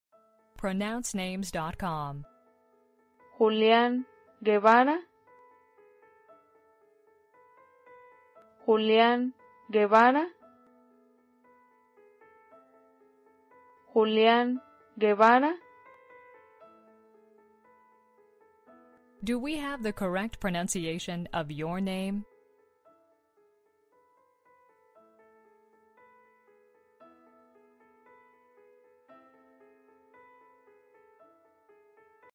How do you say Julian Guevara in Mexico (Mexican Spanish)? - PronounceNames.com
How do you pronounce Julian Guevara in Mexico? For more information about Julian Guevara, such as gender, origin, etc.,